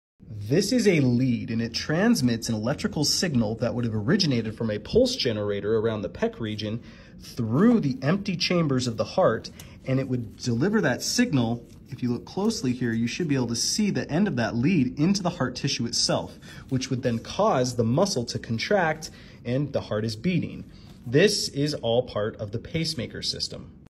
How the Pacemaker System works for the Heart
The artificial heart pacemaker system.

Shout out to I.O.H.A